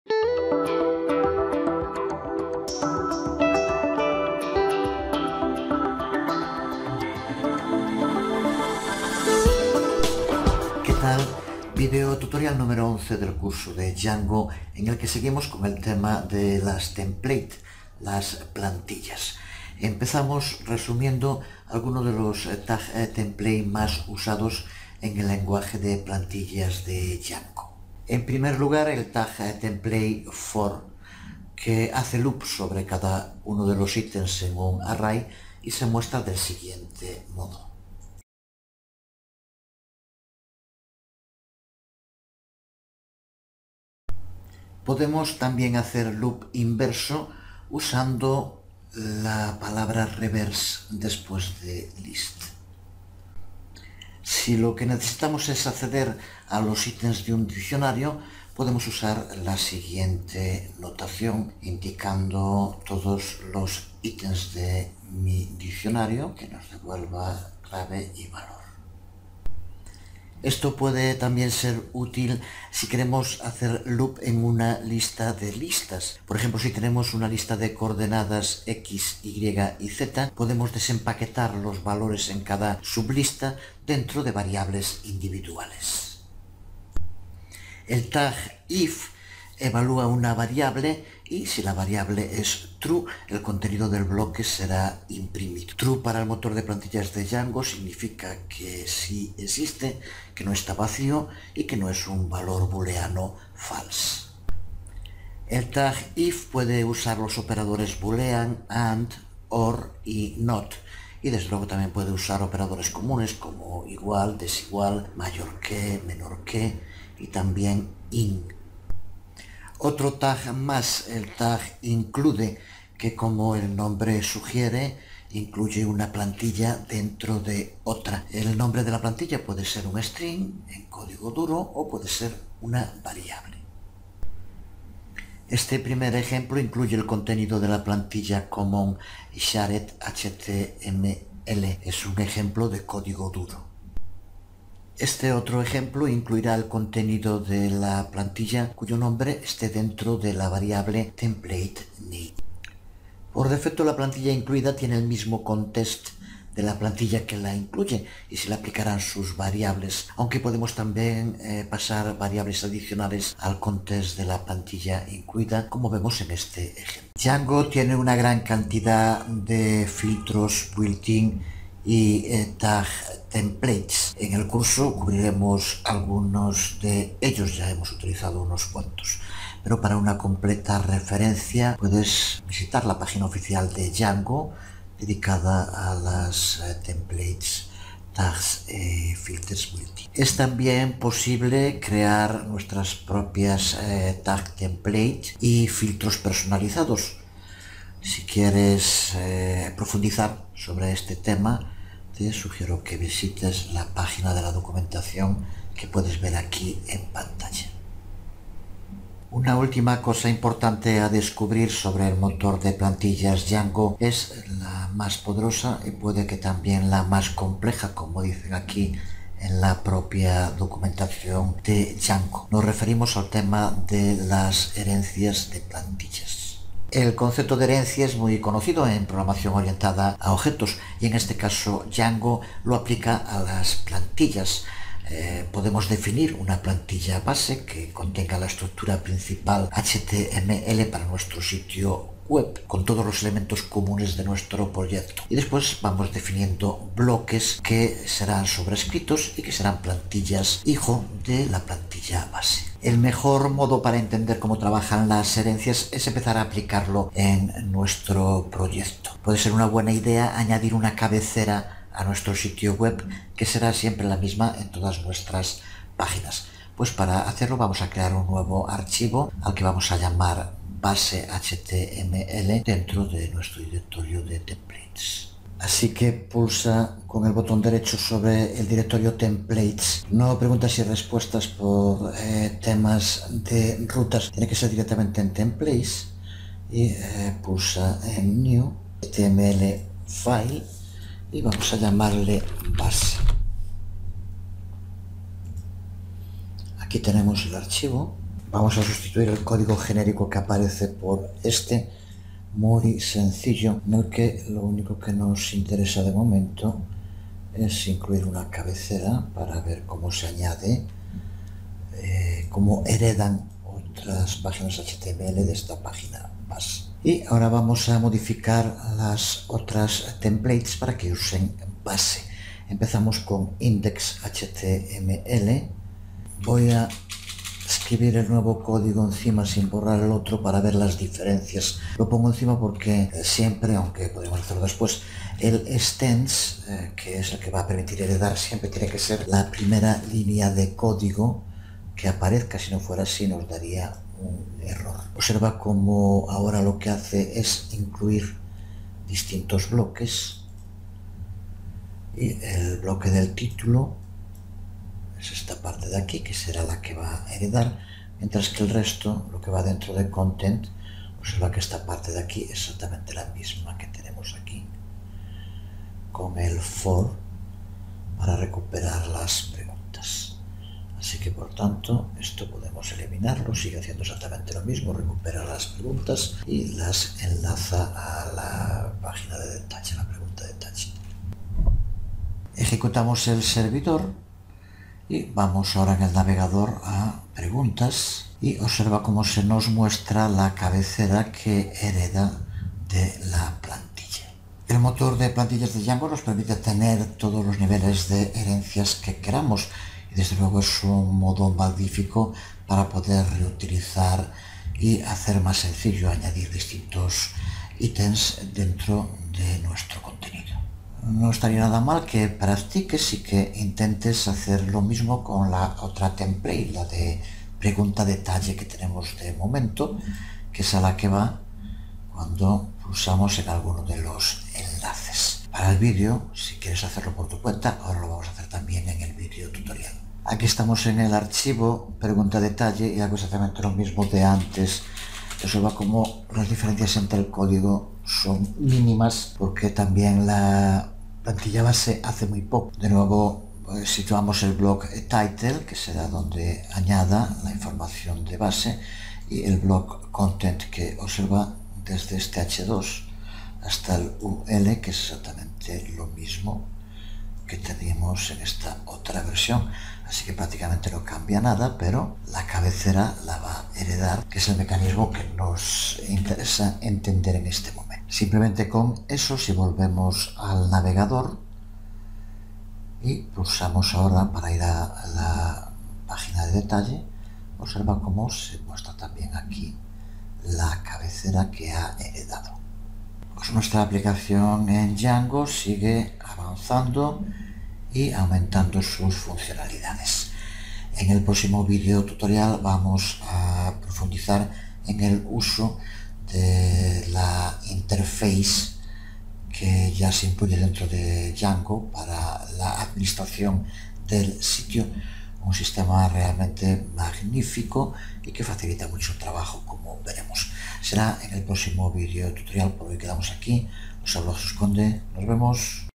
11.- Curso de Django. Imports y herencias en templates.
Videotutorial nº 11 del Curso de Django, en el que seguimos con las Templates. Vemos los principales tag template, los imports y las herencias. Creamos una template base de la que heredarán el resto de las templates. 9 minutos.